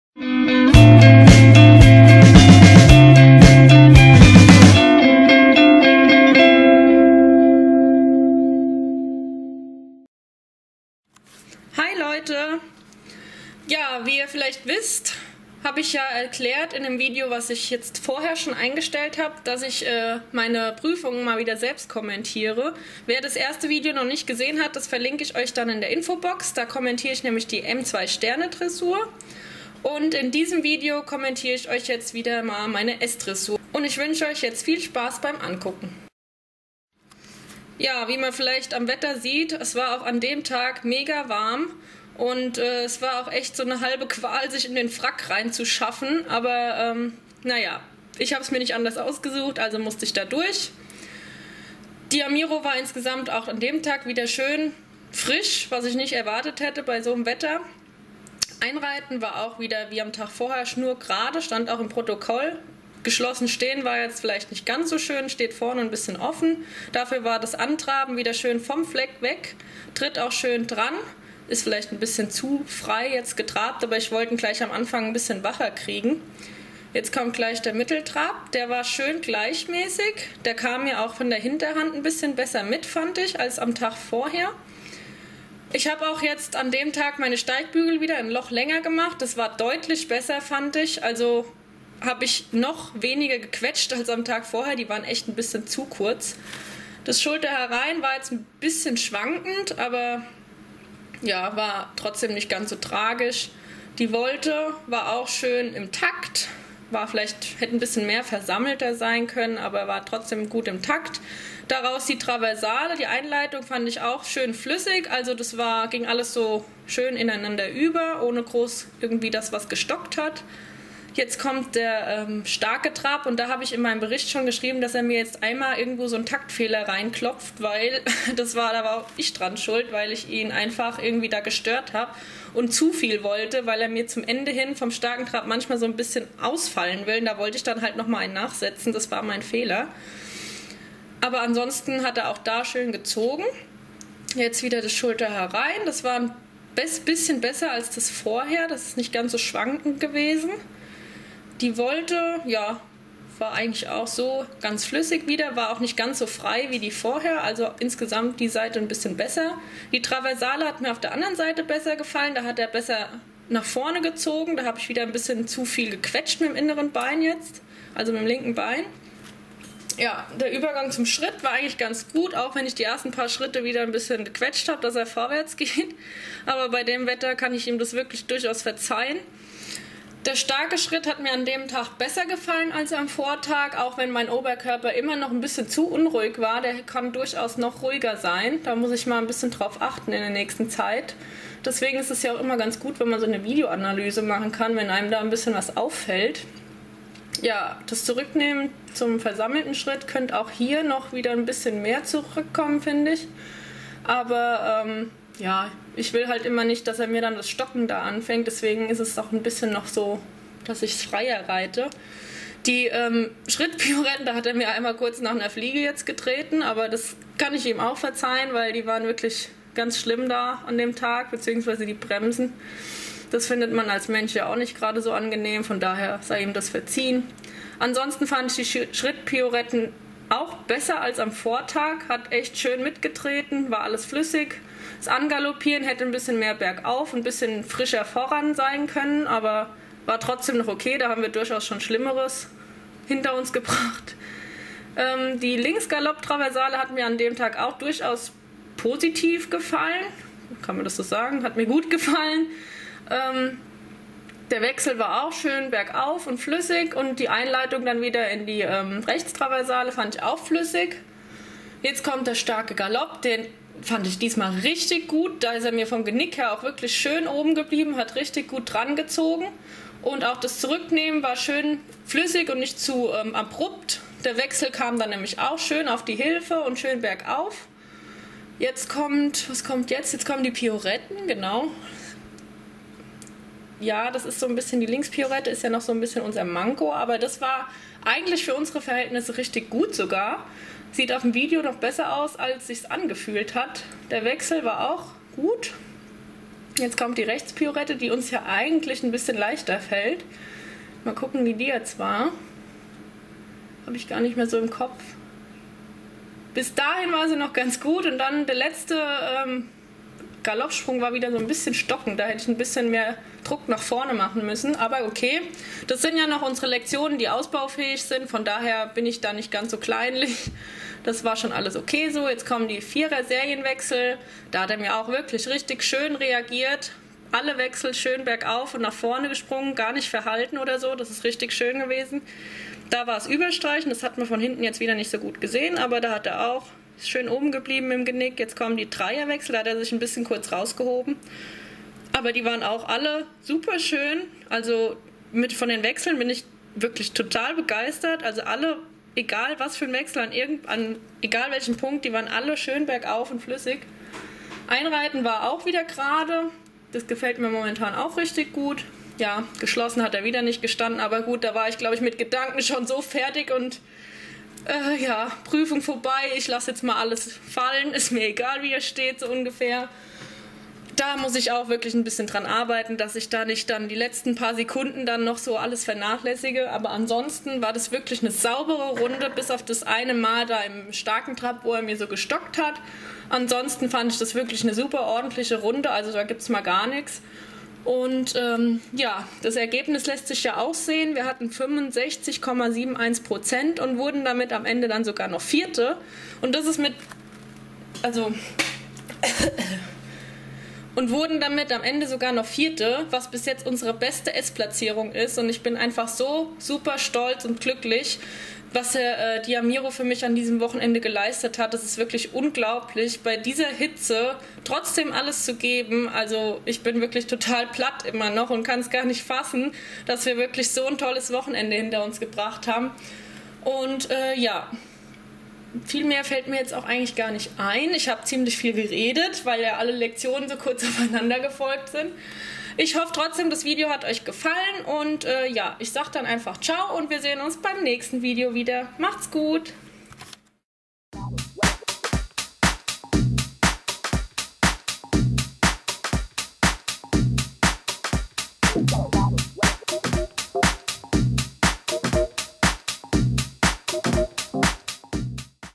S-Dressur - [Ich kommentiere meine Prüfung selbst]
Hier nun der "zweite Teil". Die kommentierte S-Dressur.